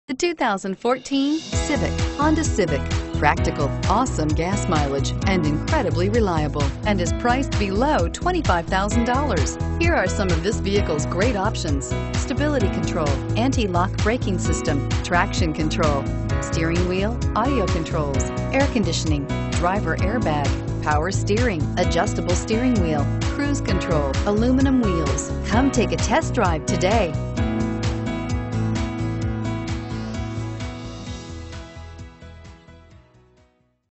2014 HONDA CIVIC SEDAN Milford, CT 20578
2014 HONDA CIVIC SEDAN Milford, CT
Stock# 20578

Courtney Honda
767 Bridgeport Ave

Don't miss this 2014 Honda Civic Sedan. It's equipped with Variable transmission and features a BK BK exterior. With 6 miles, you'll want to take this car home. Make a great choice today.